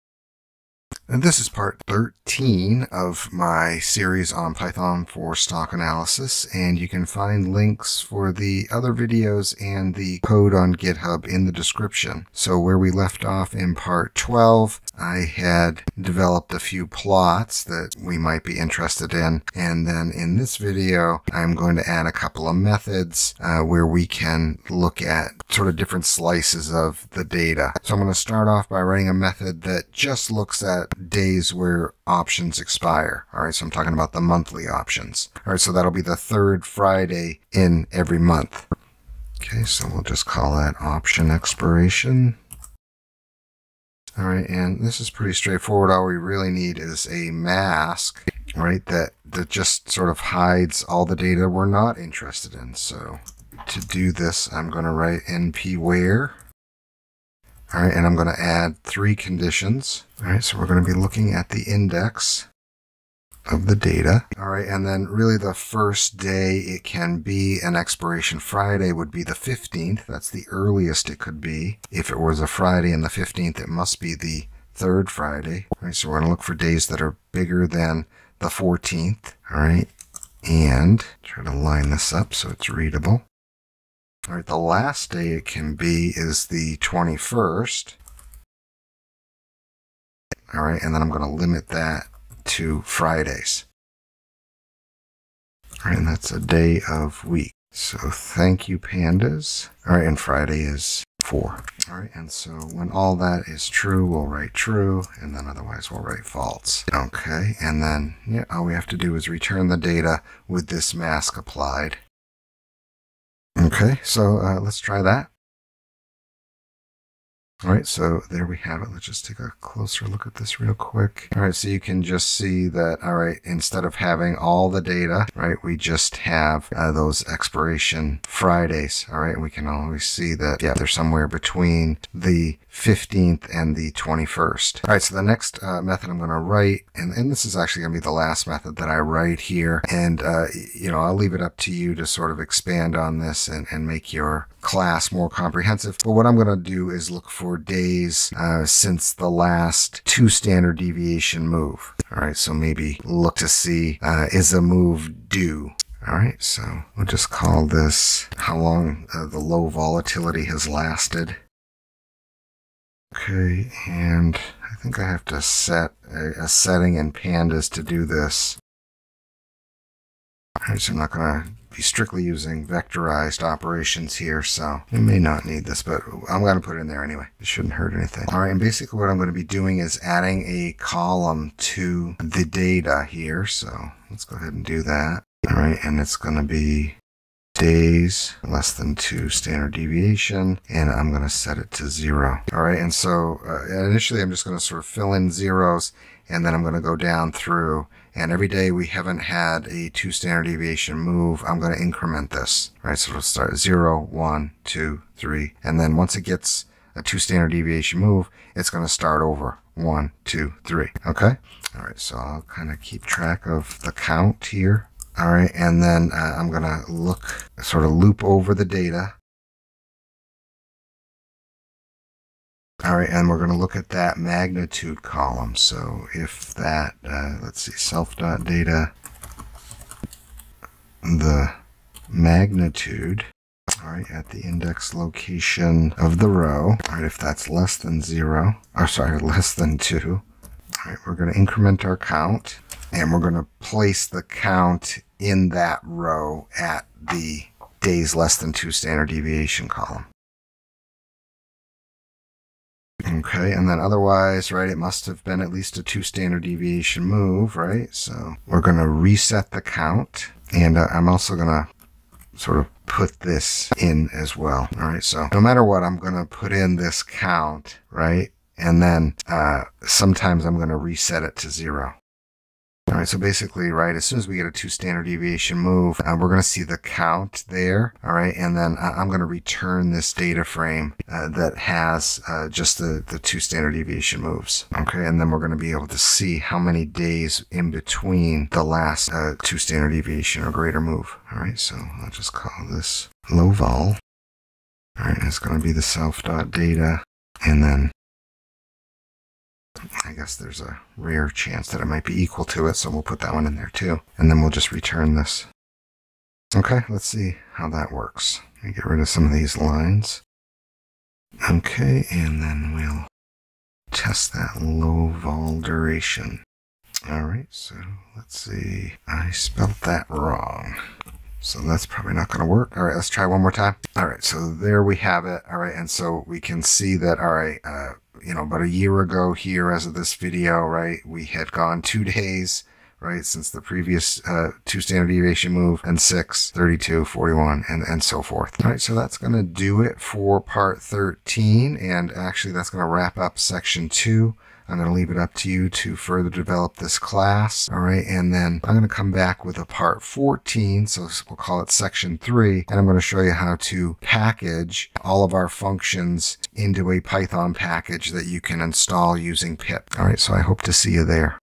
Python Pandas: Filter Time Series Data by Option Expiration & Low Volatility Duration | Part 13 📅
Welcome to **Part 13** of the Python Stock Analysis Course! This video concludes Section 2 on Individual Security Analysis by adding advanced data filtering capabilities to our custom `Stock` class.

You will learn how to create two methods that slice the stock's data, allowing you to focus your analysis only on days relevant to high-impact financial events or market conditions.

### 🎯 Key Learning Outcomes:
1.  **Option Expiration Filter:** Implement logic to create a data mask that isolates the **Third Friday** of every month, which corresponds to the main monthly **Option Expiration** date.
2.  **Date Filtering with Pandas:** Utilize **NumPy's `np.where`** and **Pandas' DatetimeIndex properties** (day and weekday) to accurately define the expiration mask.
3.  **Low Volatility Duration:** Write a non-vectorized, iterative method to track the number of **consecutive days** since the last **Two Standard Deviation (2-